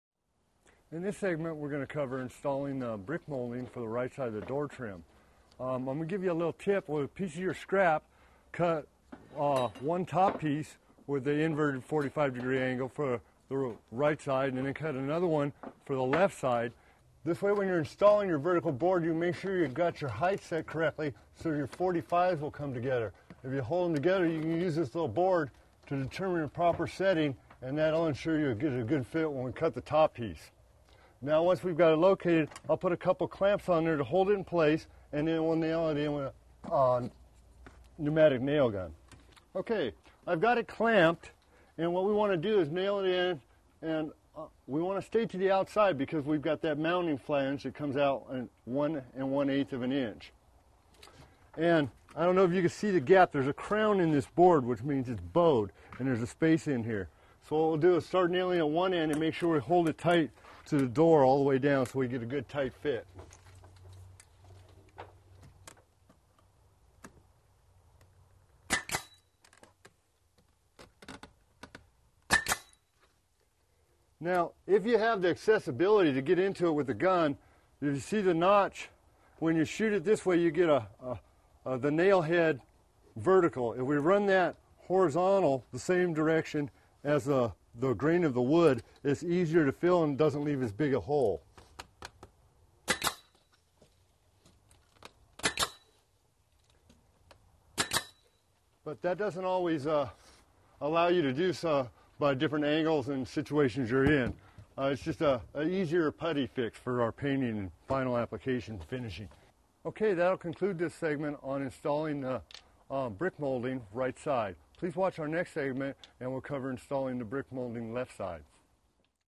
Replacing Sliding Glass Doors: Installing Brick Molding on Right Side
Replacing Sliding Glass Doors: Installing Brick Molding on Right Side. Part of the series: Replacing Sliding Glass Doors. When installing brick molding, use scrap to make sure the height is set correctly. Install brick molding for sliding glass doors with tips from a certified home inspector in this free video on home repairs and maintenance.